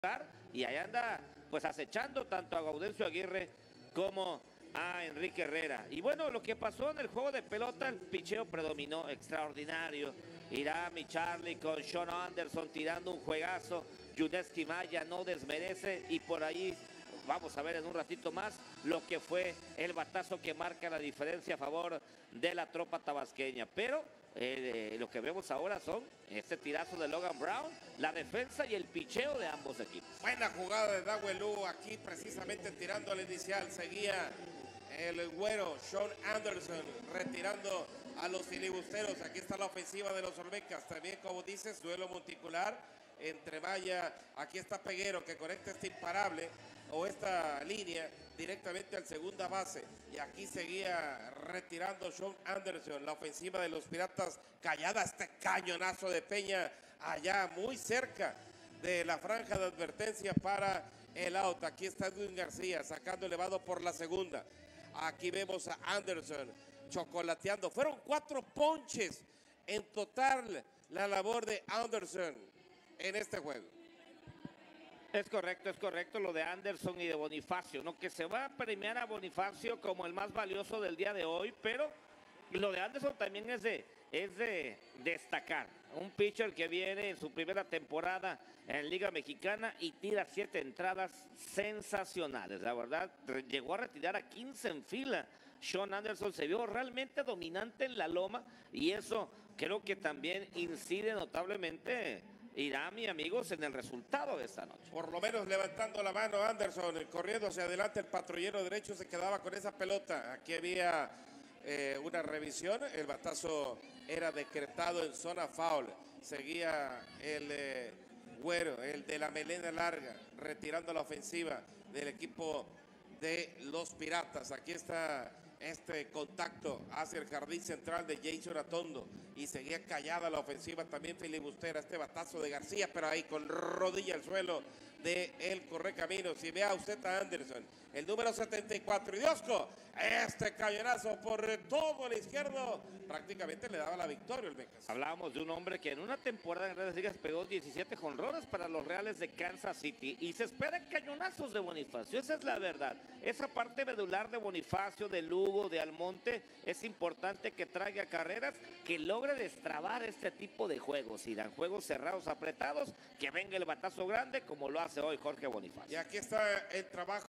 Olmecas se lleva el encuentro por la mínima ante Piratas (13/04/2024)
Highlights del juego entre Olmecas de Tabasco y Piratas de Campeche